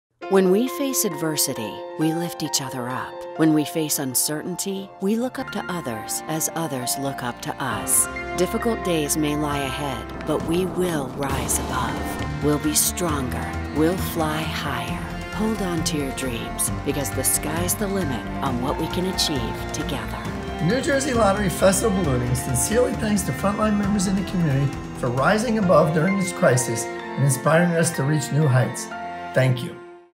Our friends at WABC and the NJ Lottery Festival of Ballooning send THANKS to all essential workers!
Our friends at WABC and the New Jersey Lottery Festival of Ballooning send THANKS to all essential workers!!!